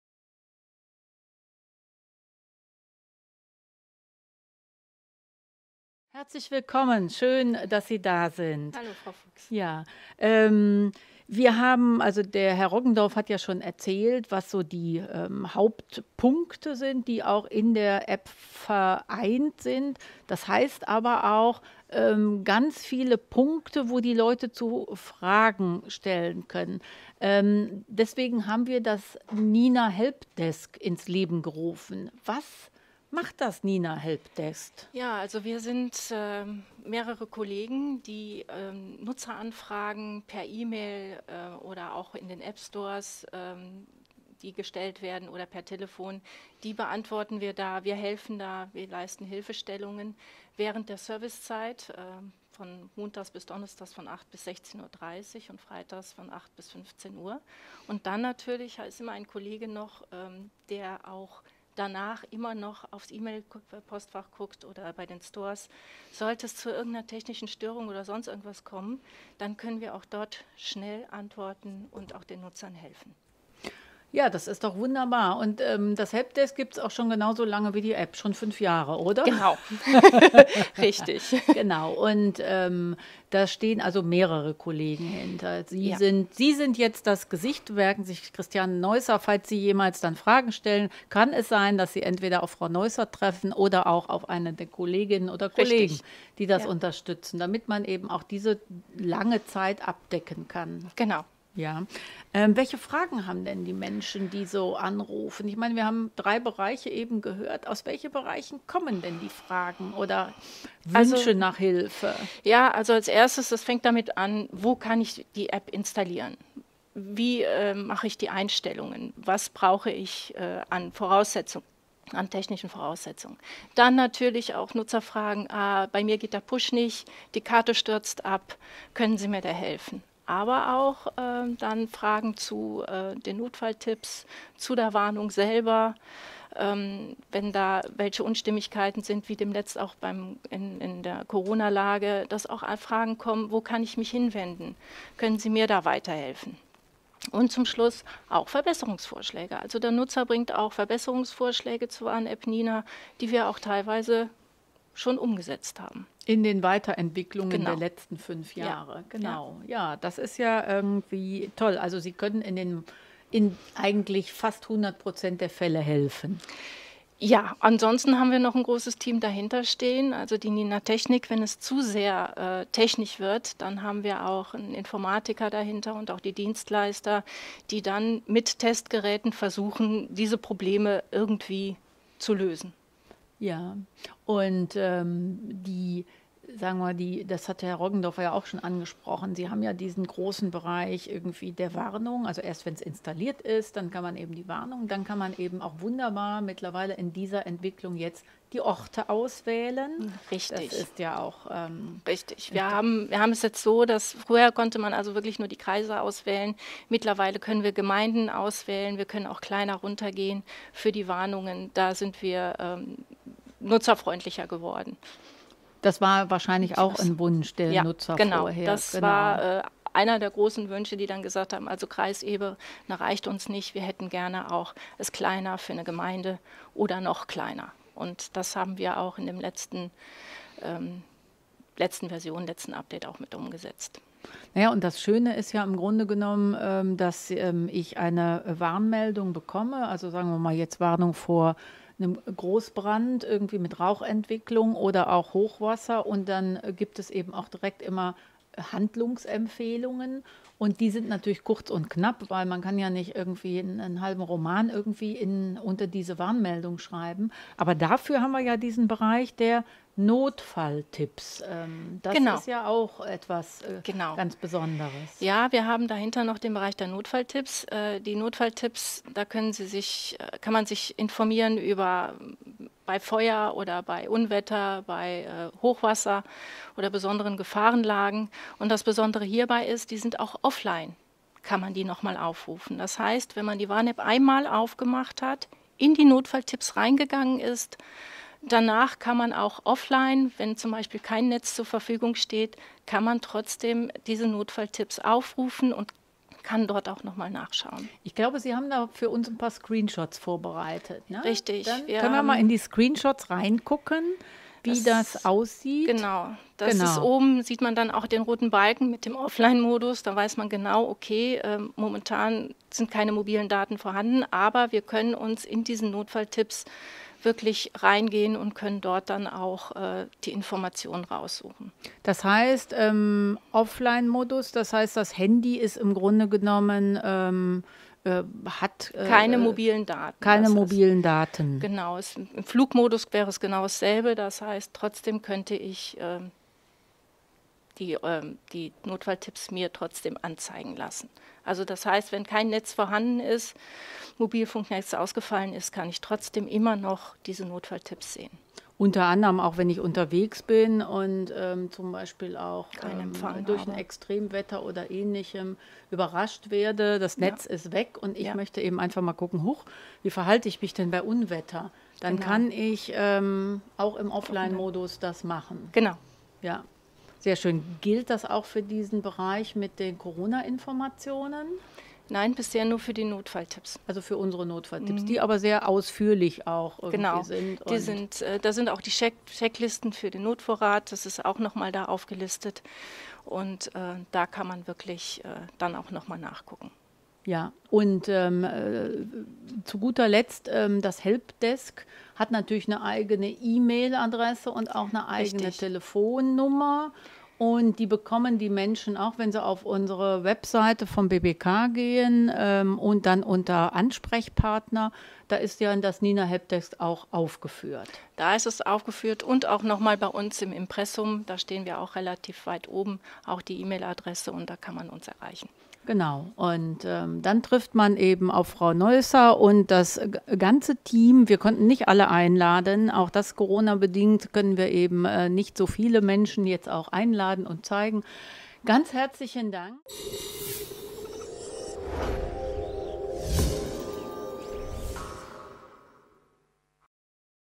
BBKvirtuell: NINA - Warn-App und Helpdesk
NINA beim virtuellen Tag der offenen Tür: Christiane Neußer vom Referat Warnung der Bevölkerung des BBK (Bundesamt für Bevölkerungsschutz und Katastrophenhilfe) spricht am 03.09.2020 mit Ursula Fuchs über die Warn-App NINA. Dabei stellt sie besondere Funktionen der App vor und gibt Einblicke in die Arbeit des NINA-Helpdesks.